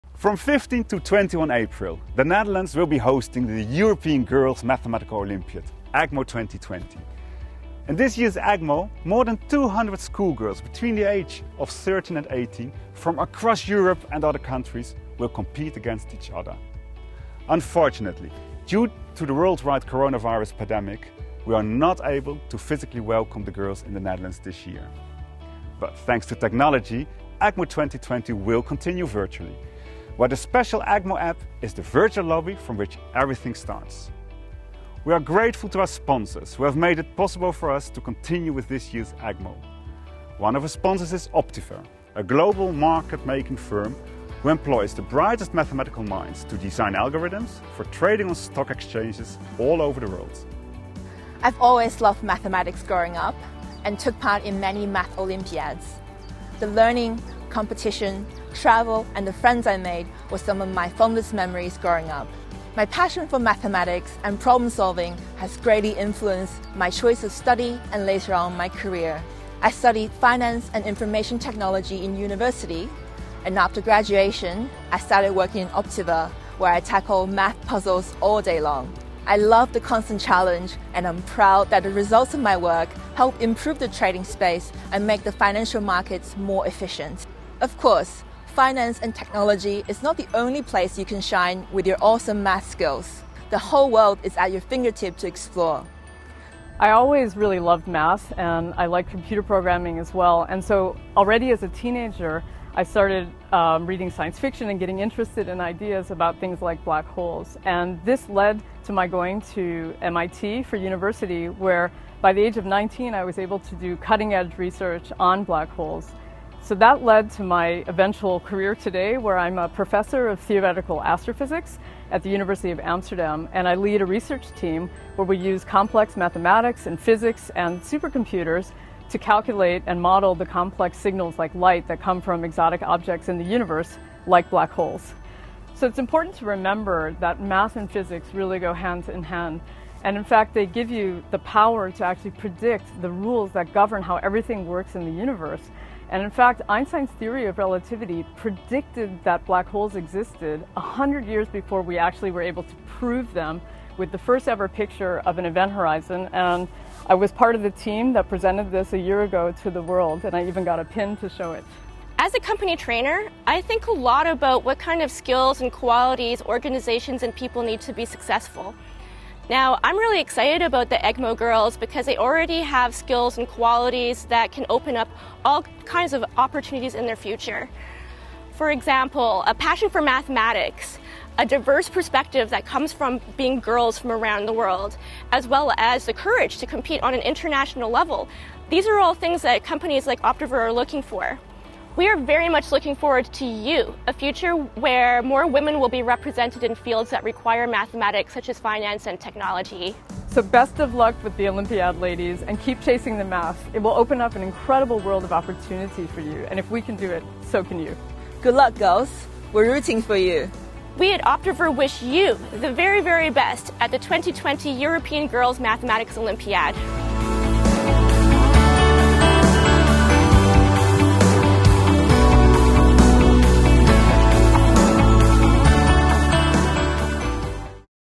2020 European Girls' Mathematical Olympiad
The 2020 European Girls' Mathematical Olympiad kicks off virtually on the 15th of April and Optiver is proud to be a sponsor.
We want to wish everyone competing the best of luck! Keep following your passion for mathematics and a world of opportunity will open up to you!